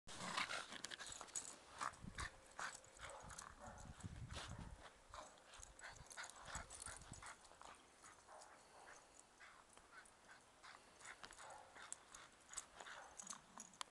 Rascal playing in the snow.
Since his ball won't roll in the snow the way he likes, Rascal carries it back and forth across the yard.